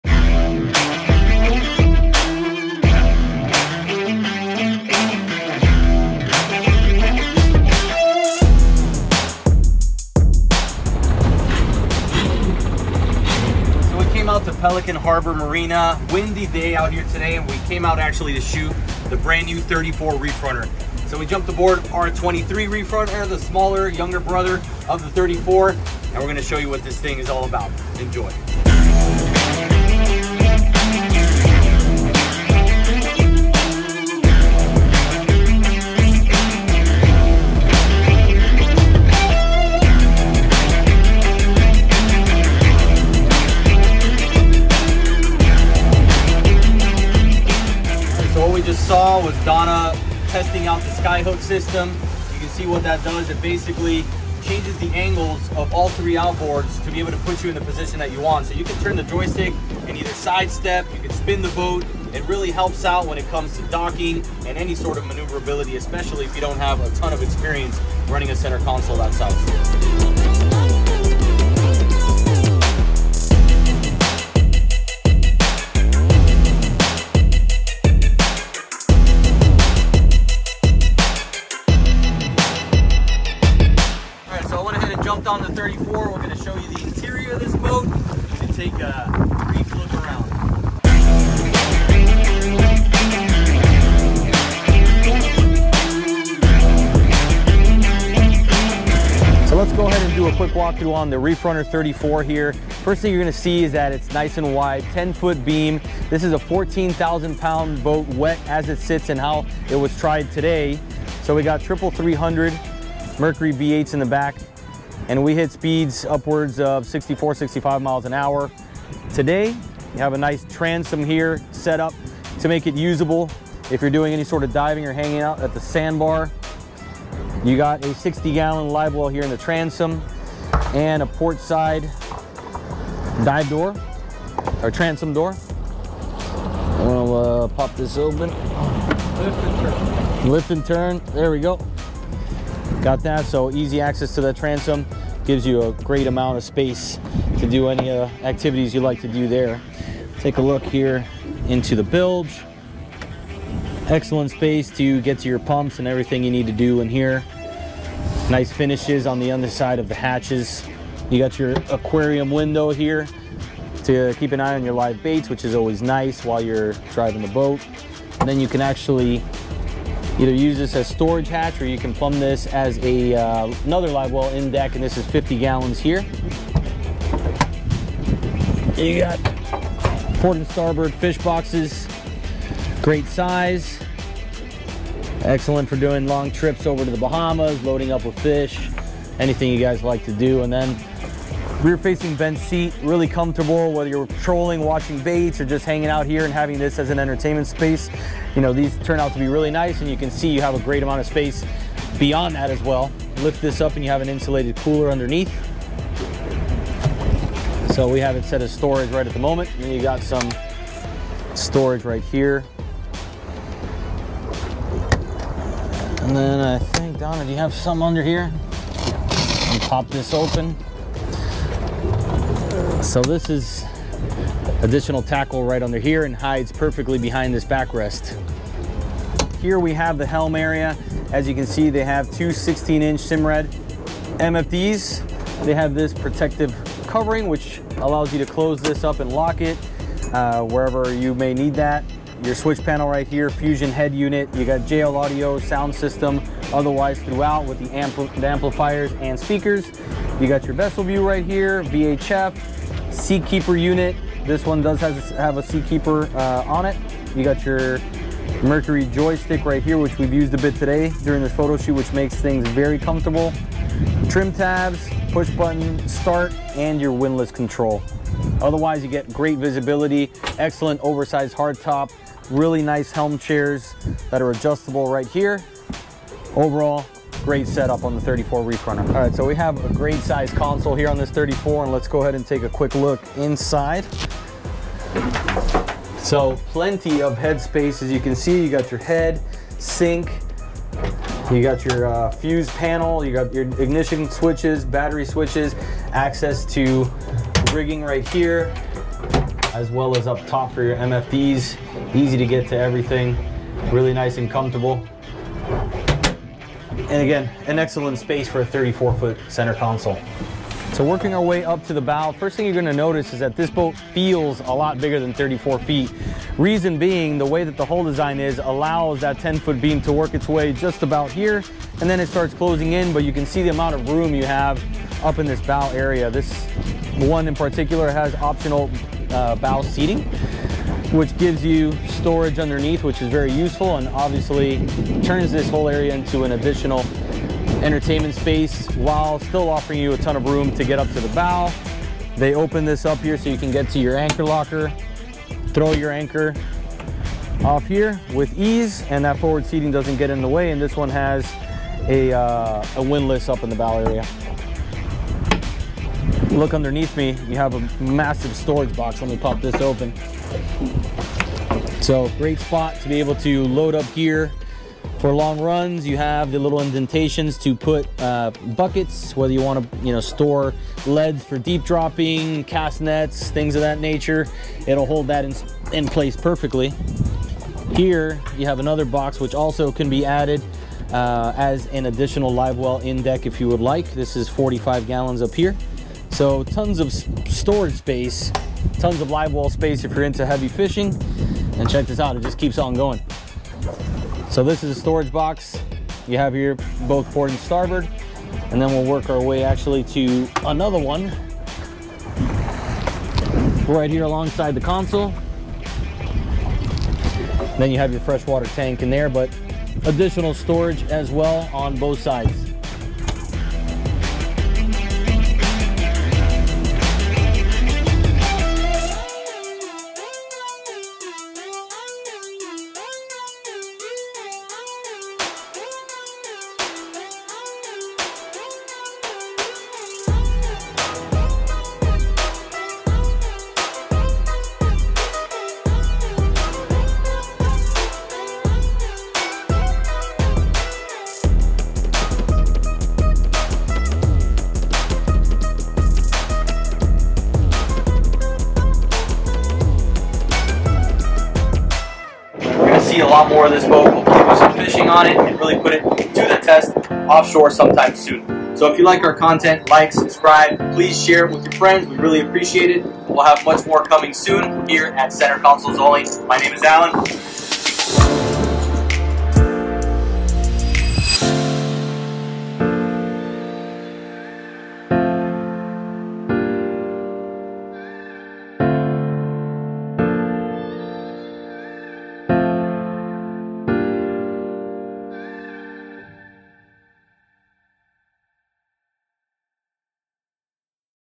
Reef Runner NEW 34!
We had a chance to jump on the 34 Reef Runner for the first time since we sea trialed it at Lake X. This boat really impressed us can't wait to take it fishing.